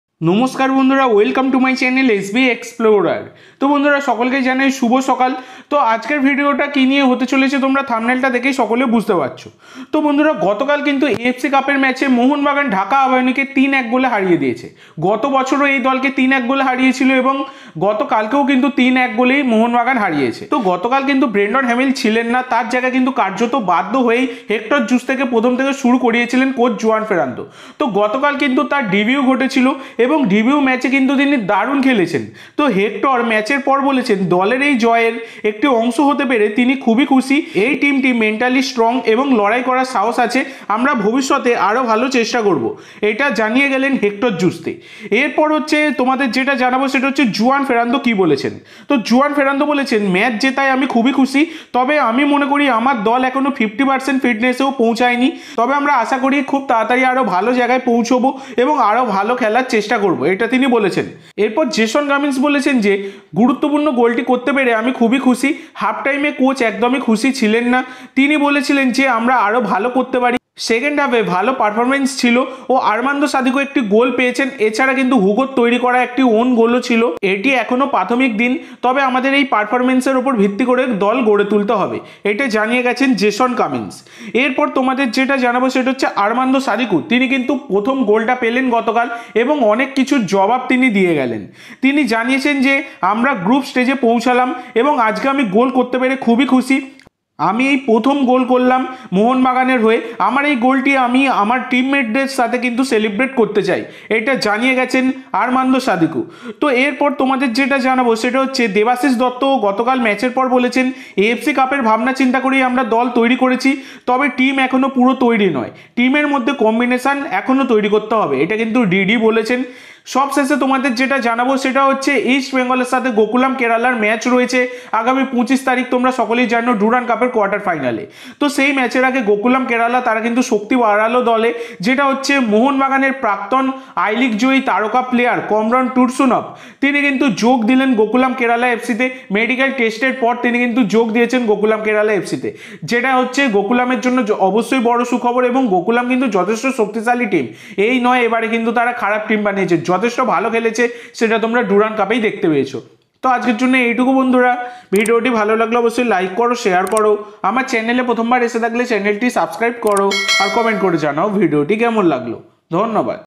🚨 আবাহনীকে হারিয়ে বড়ো বার্তা জেসন ও জুয়ানের🤩জবাব দিলেন সাদিকু🔥যোগ দিলো কমরন😱|MBSG|Mohunbagan
Comment ⌨️ করুন।

ধন্যবাদ🙏🙏🙏🙏🙏